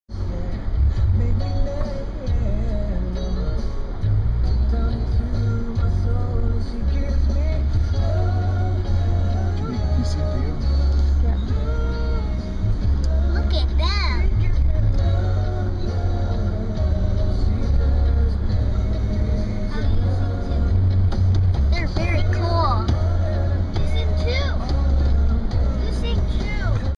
The Shark Boys
Theo and Grandpa formed a Band while in Hawaii, which Theo named "The Shark Boys".  Here's one of their jam sessions.